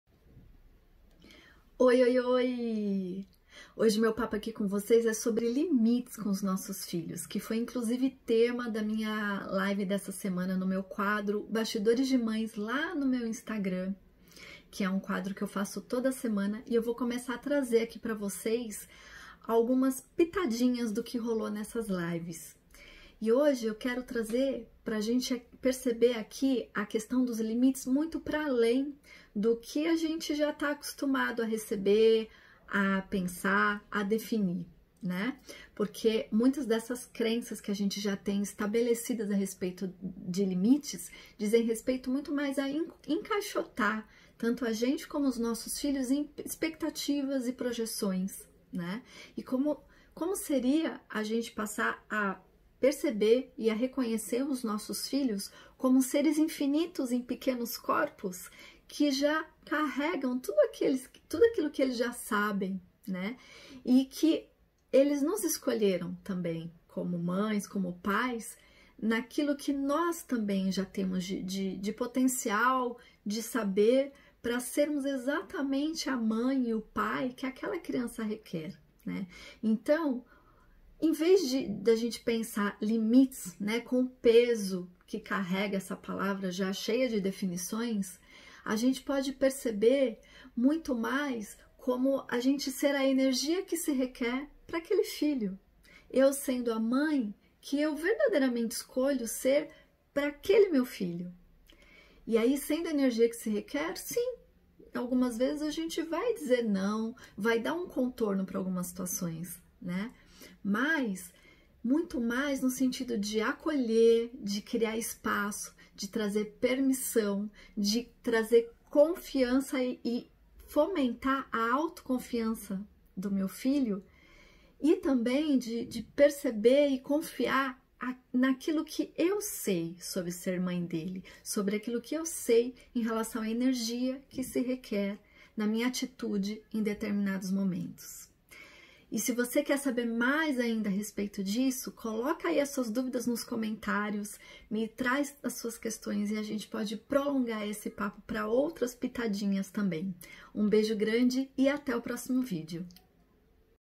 Nossos filhos precisam de limites?
Neste vídeo, eu trago uma "pitadinha" sobre o que rolou nos BASTIDORES DE MÃES, um quadro de lives que eu faço com convidadas no meu canal do Instagram.
O tema dessa semana foi LIMITES. E a perspectiva que eu tenho para lhe trazer pode ajudar você a ir além de tudo o que já recebeu como conceito e definição, ou sobre certo e errado, a respeito de limites.

Me conte nos comentários o que achou desse vídeo e quais outros temas você gostaria de me ver falar aqui.
Quer ver a live completa? Voa lá no meu canal do Instagram e confere a live "Limites ou Possibiidades?" na minha série de lives "Os Bastidores de Mães".
Gostaria de saber mais sobre mim, sobre meus cursos e workshops e me acompanhar em meus outros canais, entre nesse link: